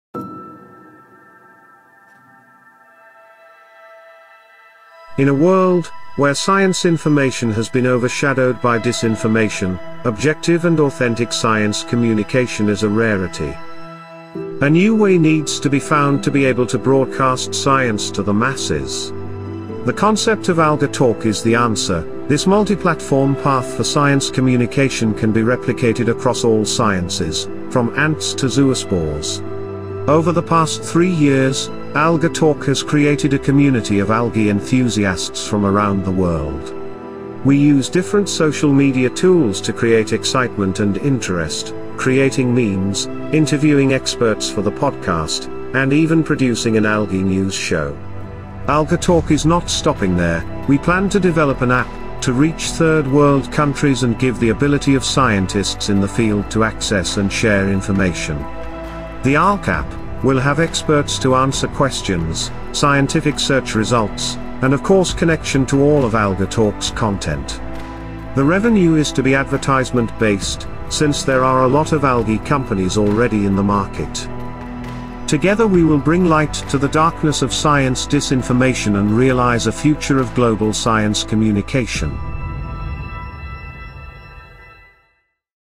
Why AlgaTalk? submission for the Forbes/Aviram Awards—Tech for Humanity Competition
AlgaTalk is a podcast originally started in 2018 by two co-workers and algae enthusiasts. The concept came from a lack of finding algae-based content. There was no podcast, youtube channel, or organization dedicated to algae public outreach to the average Joe. As an example, StarTalk, where AlgaTalk gets its name, was started by a content creator, Neil DeGrasse Tyson, who produced content on a specific scientific subject across multiple social media platforms.

In a world where science information has been over-shadowed by dis-information, objective and authentic science communication is a rarity.A new way needs to be found to be able to broadcast science to the masses. The concept of AlgaTalk is the answer, this multi-platform path for science communication can be replicated across all sciences, from Ants to Zoospores.

Algae is often seen by the public in a negative light, on their local news about toxic algae blooms, or growing in their pools or aquariums. The public rarely gets to see the beauty of algae, most don’t know it’s in the oxygen they breathe, in the toothpaste they use, or the ice cream they eat. And the future is bright for algae, being high in fatty-lipids it is being researched for uses in biofuels, plastics, and is even being researched in space on the International Space Station.

Over the past 3 years, AlgaTalk has created a community of algae enthusiasts from around the world. We use different social media tools to create excitement and interest, creating memes, interviewing experts for the podcast, and even producing an algae news show. We have seen over 7,000 downloads of the podcast, have reached almost 2,000 followers on Twitter and Instgram respectively; and have reached over 10,000 page likes on Facebook. Our most recent creation, the AlgaTalk YouTube channel, has around 100 subscribers and average over 100 views.

Our statistics tell us that the majority of our fans are from the United States of America and are connected to algae in some way. We believe this is the first stage of creating global awareness, connecting the global community of a certain science. This is the first step, next we hope to see family members and friends start to follow AlgaTalk. The last stage would be the science information being in the ether of the global culture and fun facts about algae being known by the general public. This will, in theory, raise the algae industry to new heights of profitably and acceptance. All of this will be fed by the public’s demand for environmental and greener solutions to today’s problems.

AlgaTalk is not stopping there, we plan to develop an app that will allow easy access by mobile phone, to reach third world countries and give the ability of scientists in the field to access and share information. The AlgaTalk app, AlgApp, will be a one stop shop for anyone’s algae needs. It will have experts to answer questions, scientific search results, and of course connection to all of AlgaTalks content. The revenue to run the app is planned to be Ad based, since there are a lot of algae companies already in the market.

Together we will bring light to the darkness of science dis-information and realize a future of global science communication.